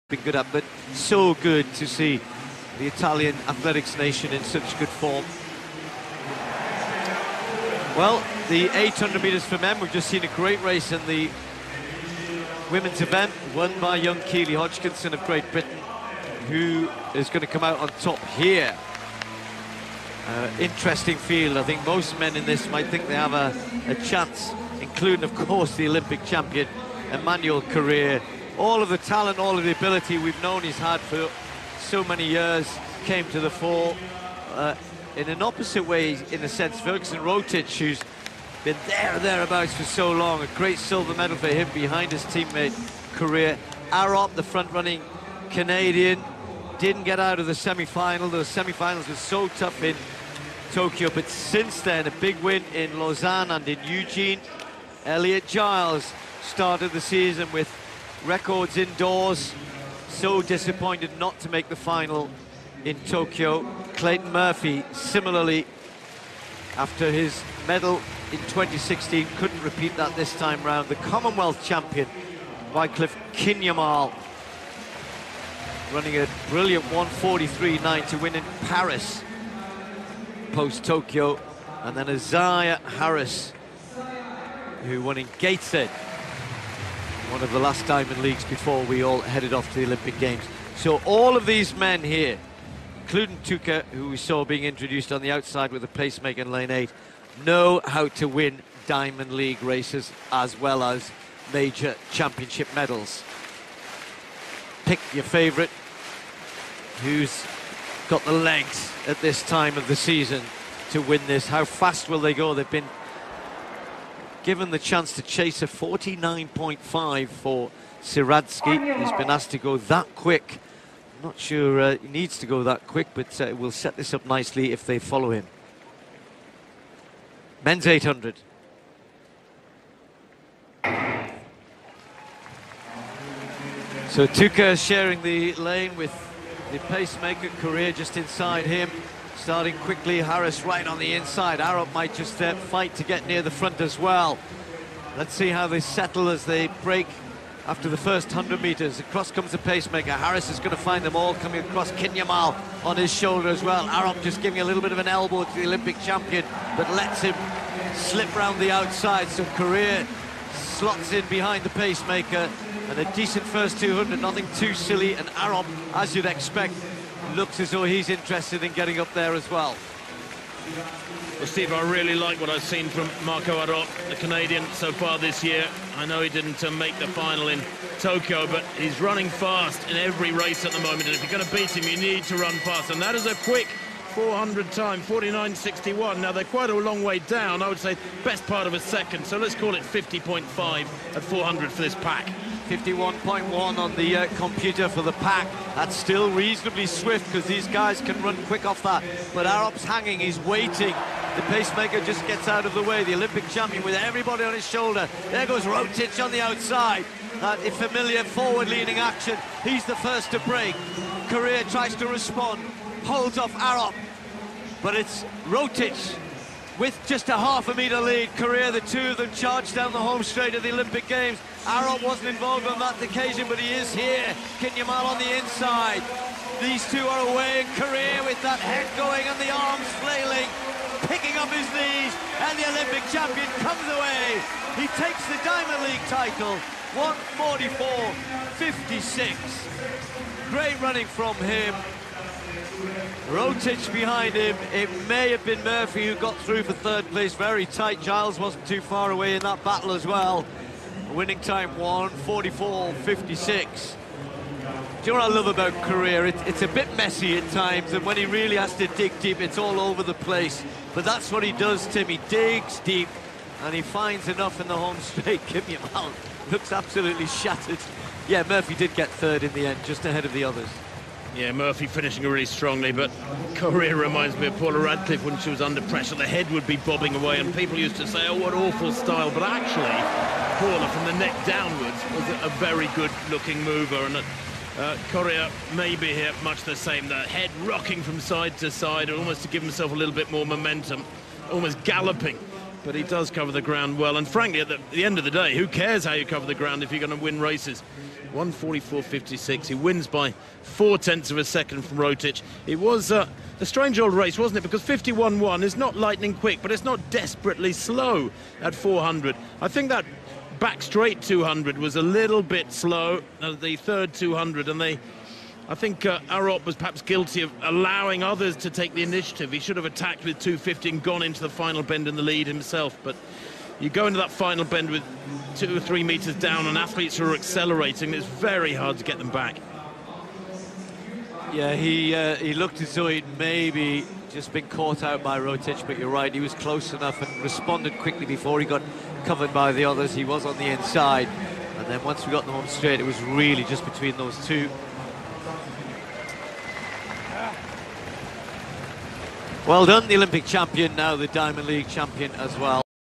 Men's 800m Final. Weltklasse Zürich Diamond League, Day 2, Letzigrund Stadium, September 9, 2021.
Up next for the Athletics Fans at Zürich's Letzigrund Stadium is the Men's 800m Final, featuring a complete Line Up of Experienced Diamond League Men's 800m event winners, including the Olympic Champion Emmanuel Korir (Kenya), and the Olympic Silver Medalist Ferguson Rotich (Kenya).
At the end the Olympic Champion Korir proved too strong  for his compatriot Rotich by Four 10ths of a Second, and got to take home the 2021 Diamond League Trophy and the US$30,000 as winner of the Men's 800m Final.

The Competitors in the Men's 800m Final and their Finishing Times:

Emmanuel Kipkurui Korir (Kenya), 1:44.56
Ferguson Cheruiyot Rotich (Kenya), 1:44.96
Clayton Murphy (USA), 1:45.21
Marco Arop (Canada), 1:45.23
Eliot Giles (Great Britain), 1.45.25
Isaiah Harris (USA), 1:45.70
Amel Tuka (Bosnia And Herzegovina), 1:46.19
Wyclife Kinyamal (Kenya), 1:46.52
Patryk Sieradzki (Poland, Pacemaker), DNF

Video Editing & Research by: Yours Truly.

Thank you for choosing to watch this event on my YouTube channel.